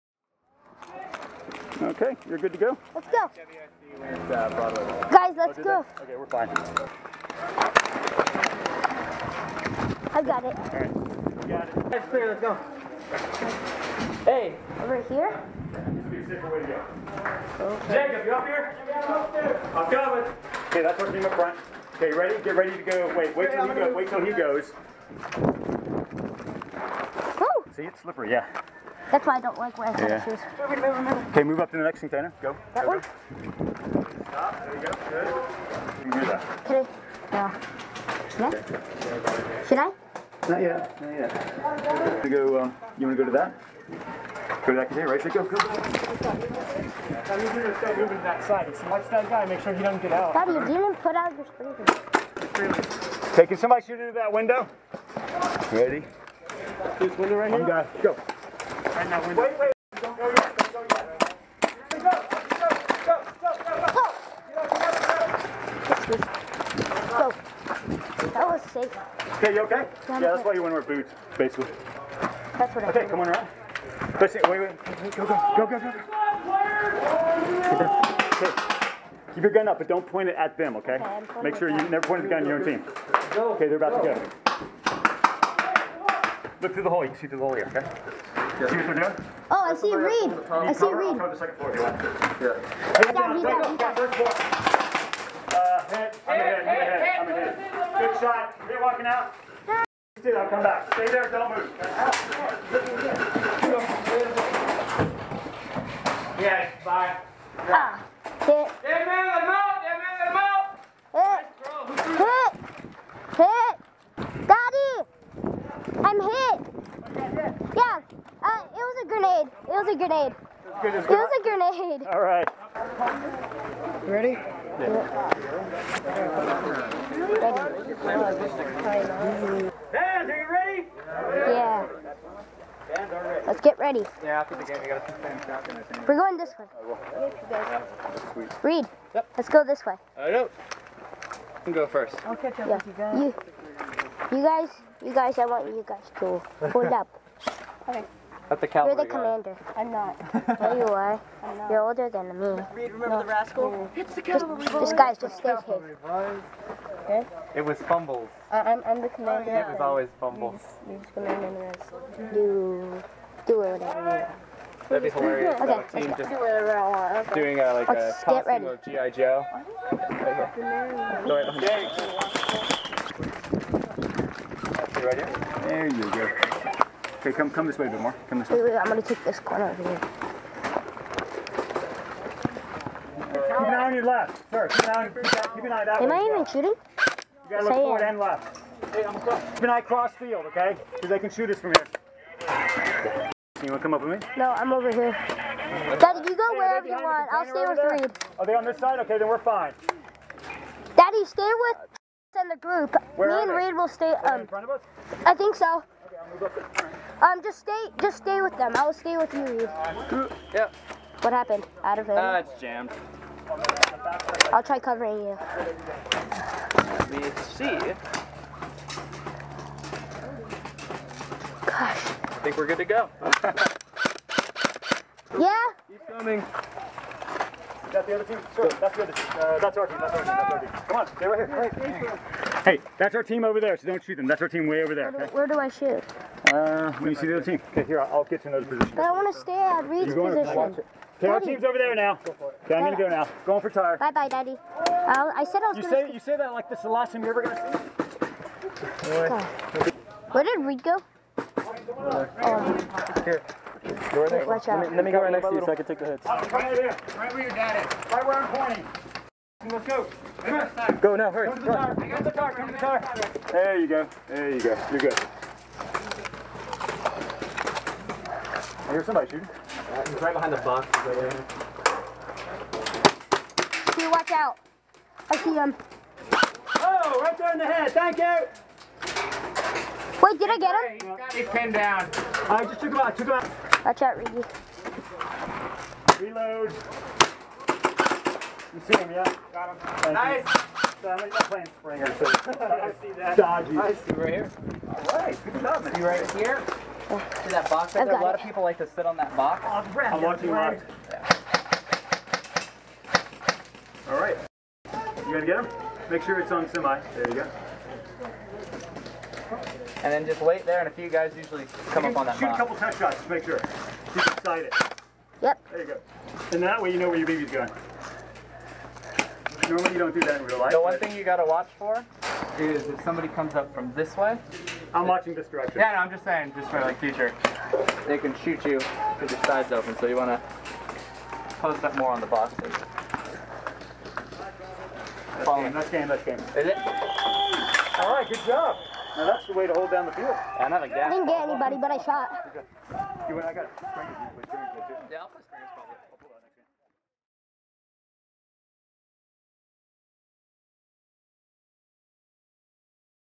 Mini-Grey First Airsoft
Vid of Mini-Grey's first day ever playing Airsoft